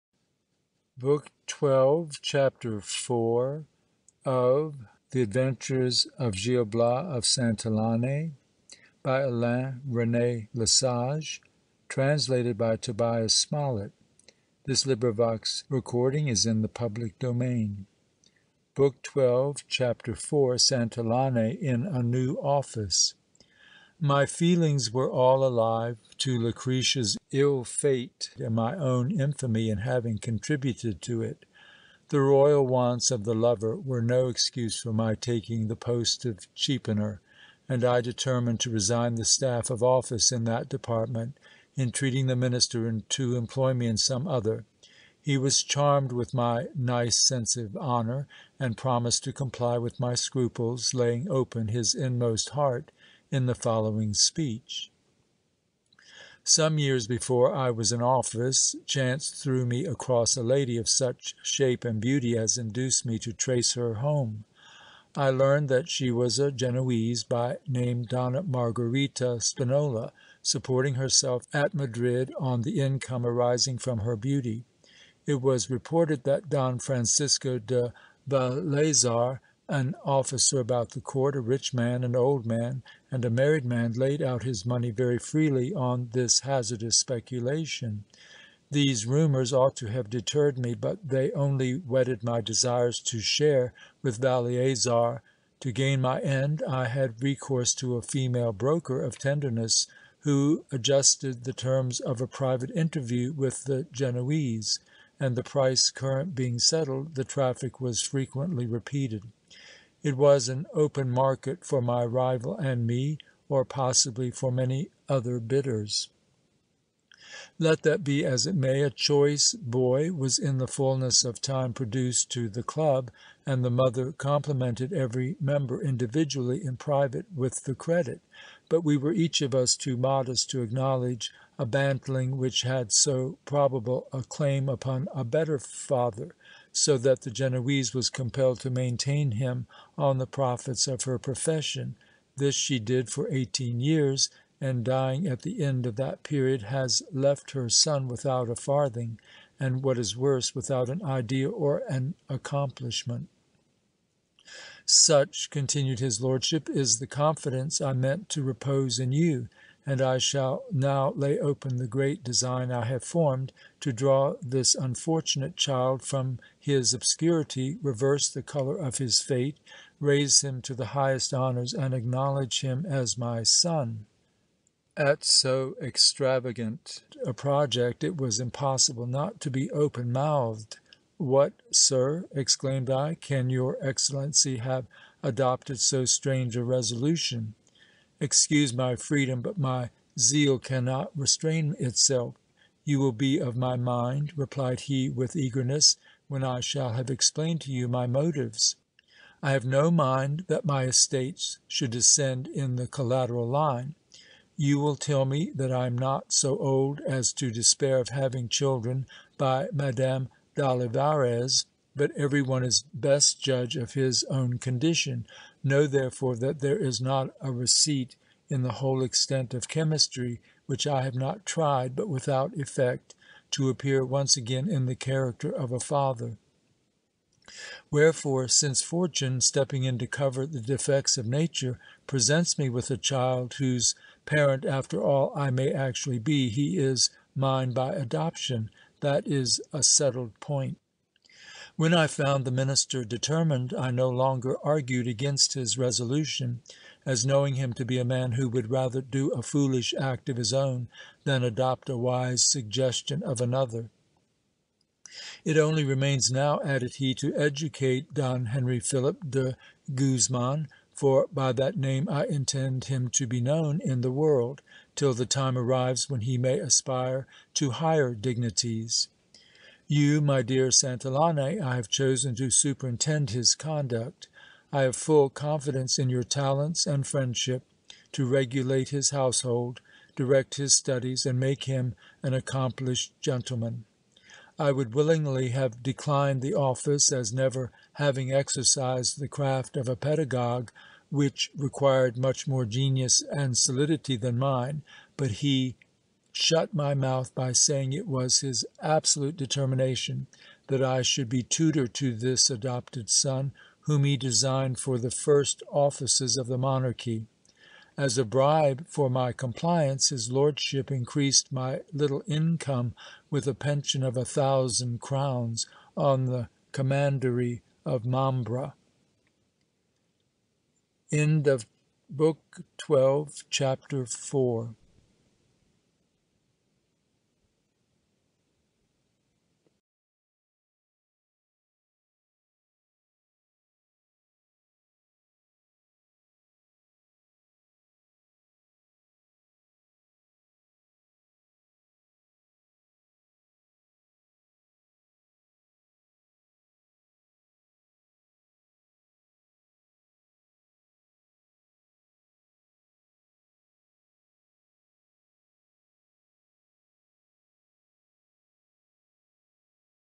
The Adventures of Gil Blas de Santillane by Alain René Lesage Book 12, Chapter 04 Full Audiobook
The Adventures of Gil Blas de Santillane

Alain René Lesage (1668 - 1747)
Translated by Tobias Smollett (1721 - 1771)

Gil Blas is born in misery to a stablehand and a chambermaid of Santillana in Cantabria, and is educated by his uncle. He leaves Oviedo at the age of seventeen to attend the University of Salamanca. His bright future is suddenly interrupted when he is forced to help robbers along the route and is faced with jail. He becomes a valet and, over the course of several years, is able to observe many different classes of society, both lay and clerical. Because of his occupation, he meets many disreputable people and is able to adjust to many situations, thanks to his adaptability and quick wit.

He finally finds himself at the court as a favorite of the king and secretary to the prime minister. Working his way up though hard work and intelligence, Gil is able to retire to a castle to enjoy a fortune and a hard-earned honest life.

Genre(s): Action & Adventure Fiction

Language: English